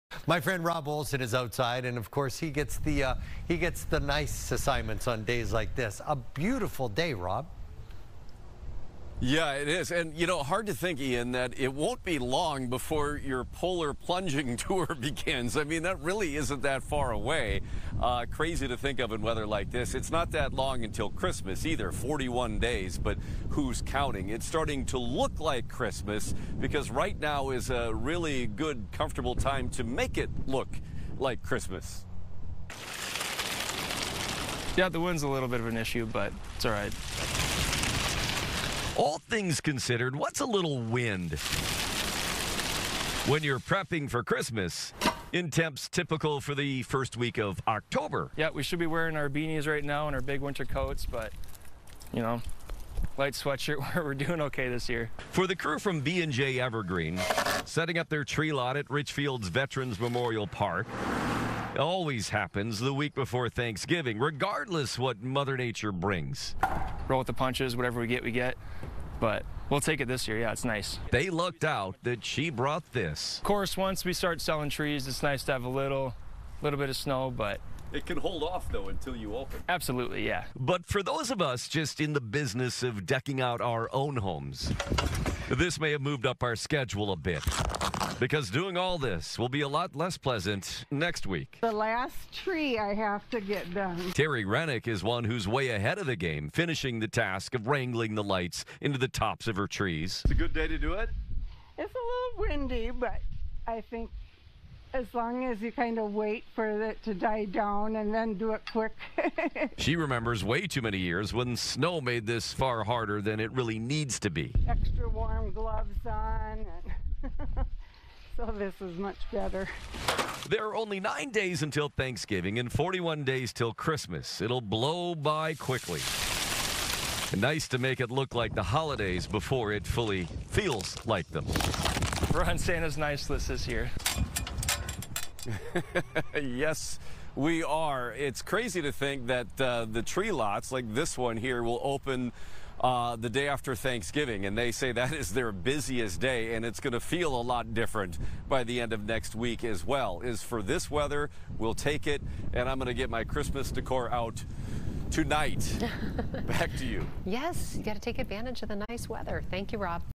Minnesotans enjoy mild fall weather
Minnesotans continue to enjoy mild weather this fall, enjoying days with temperatures in the 50s still as a not-too-distant winter approaches.